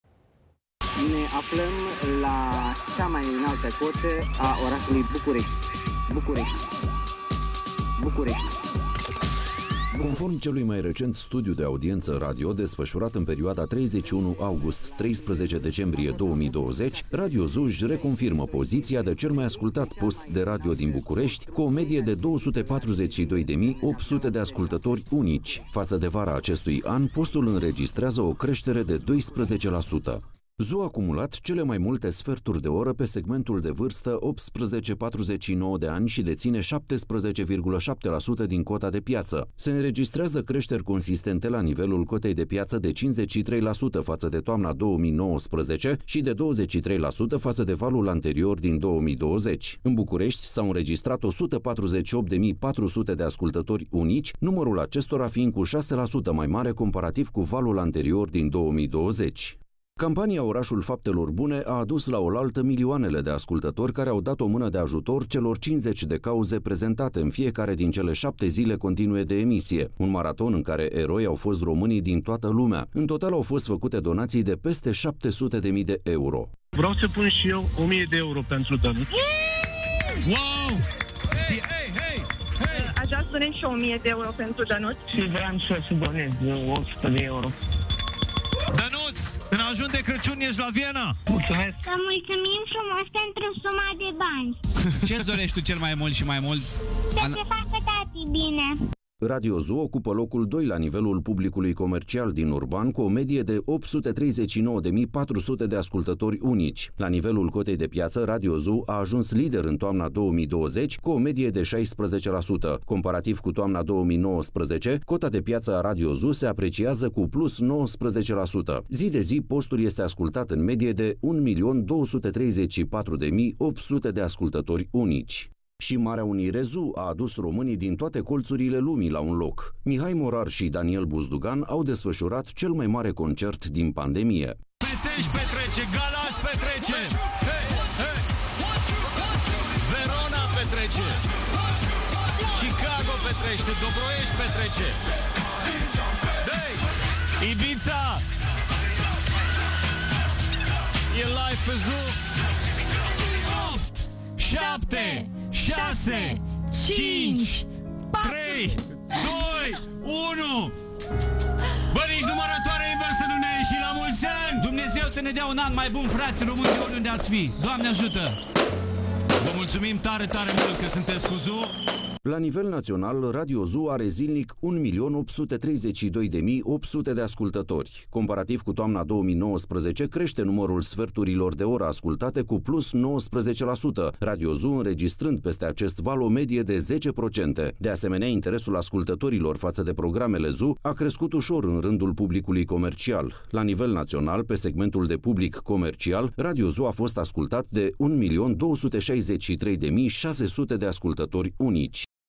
Radio ZU, pe prima poziţie în topul audienţelor
Radio ZU își menține prima poziție în topul audiențelor radio în București. Programele radioului, evenimentele pe care le-a făcut în dificilul 2020, muzica pe care a difuzat-o, toate acestea au fost apreciate de publicul care și de această dată a ales să asculte frecvențele ZU.